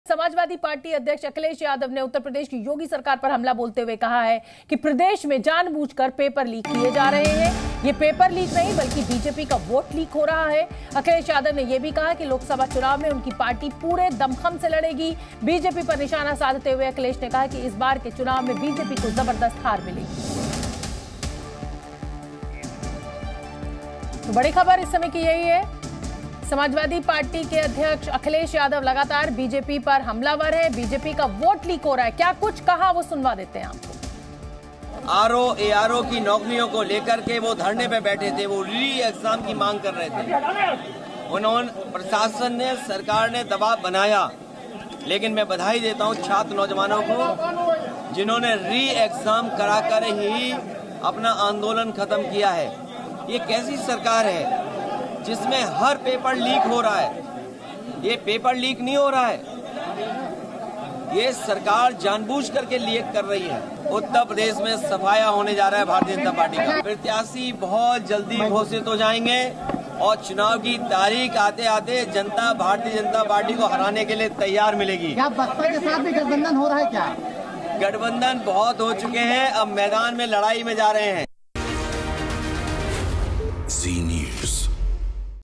Akhilesh Yadav on Paper Leak: पेपर लीक पर अखिलेश यादव का बड़ा बयान | Hindi News | CM Yogi | UP News
Akhilesh Yadav on Paper Leak: समाजवादी पार्टी अध्यक्ष अखिलेश यादव ने यूपी की योगी सरकार पर हमला बोलते हुए कहा है कि प्रदेश में जानबूझकर पेपर लीक किए जा रहे हैं.

Akhilesh Yadav on Paper Leak: Samajwadi Party President Akhilesh Yadav has attacked the Yogi government of UP and said that papers are being deliberately leaked in the state.